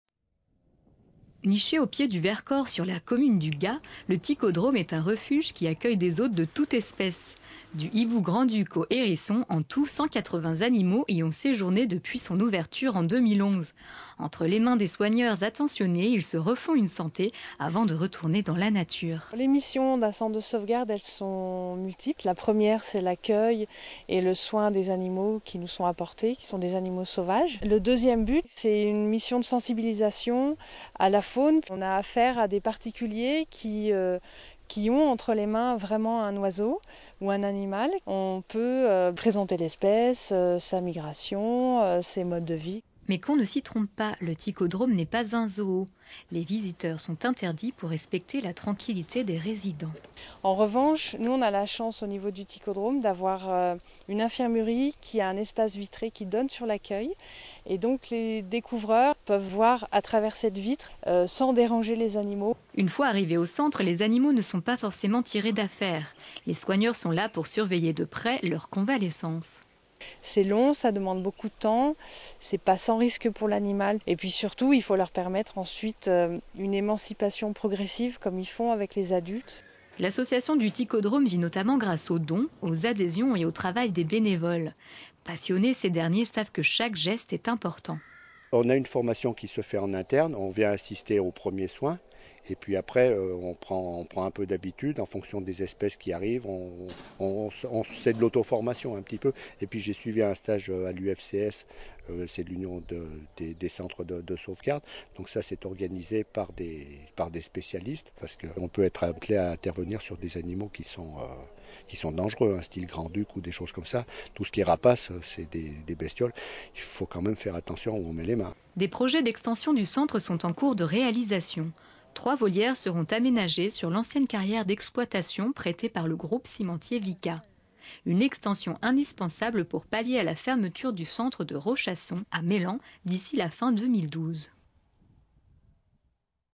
32 tichodrome icm 2mn16 avi
Reportage de Télé Grenoble sur Le Tichodrome - Centre de sauvegarde de la faune sauvage